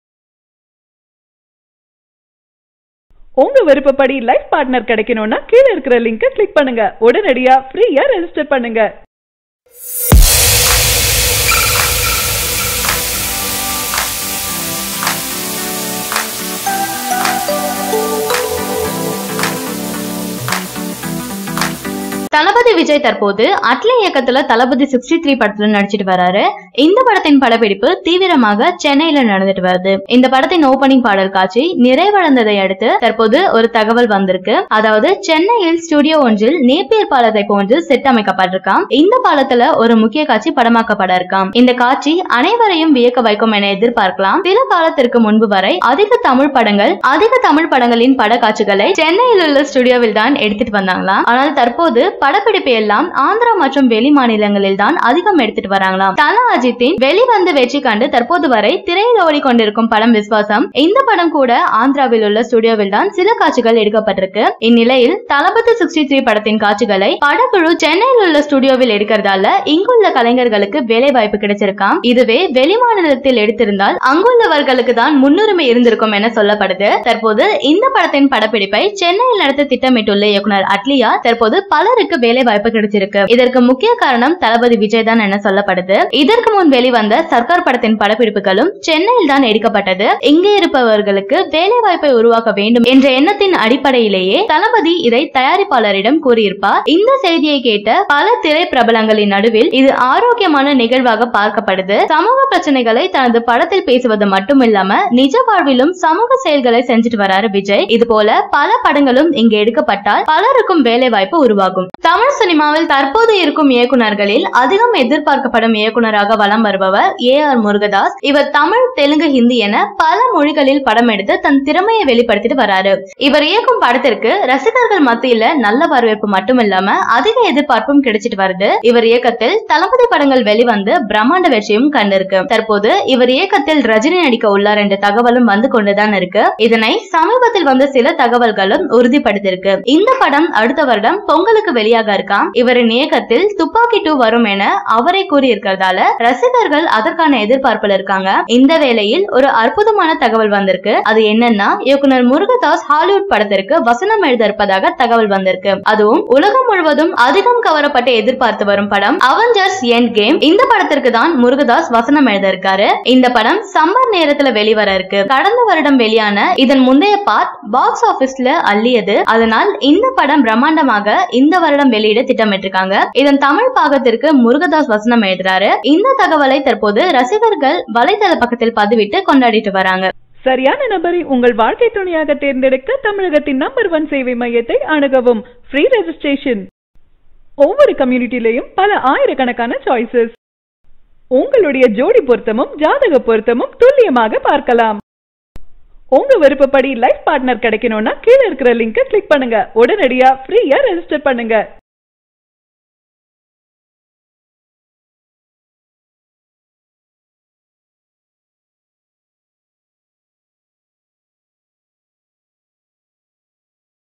AR Murugadoss Part of Hollywood Movie | Thalapathy Vijay Moment | Vijay 64
ஹாலிவுட் படத்தில் முருகதாஸ் …. வியக்க வைத்த தளபதி